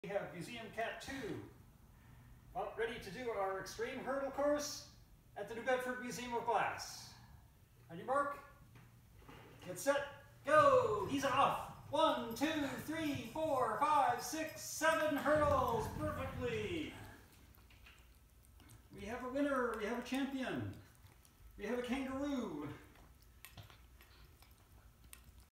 Cat Eventing Championships at the New Bedford Museum of Glass!
Extreme Hurdles: Museum Cat 2 Vaults Seven Raised Hurdles!